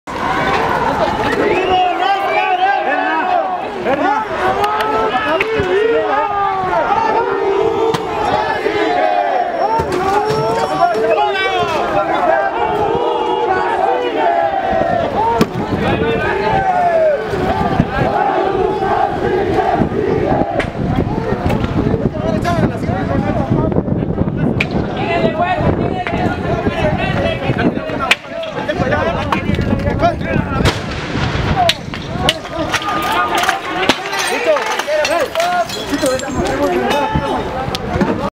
AYOTZINAPA GUERRERO 8NOV14 MVI 0101
Nov 2014 Chilpancingo, Guerrero. Jornada de protestas por los 43 normalistas desaparecidos de Ayotzinapa

VIVOS SE LOS LLEVARON...CRIMEN DE ESTADO CHILPANCINGO GUERRERO

VIVOS SE LOS LLEVARON...COMPAÑER@S DE LOS MEDIOS ALTERNATIVOS INDEPENDIENTES Ò COMO SE LLAMEN NOS ENVIAN ESTAS IMAGENES DE CHILPANCINGO GUERRERO ACCIONES POR EL CRIMEN DE ESTADO COMETIDO CONTRA LOS JOVENES NORMALISTAS!!!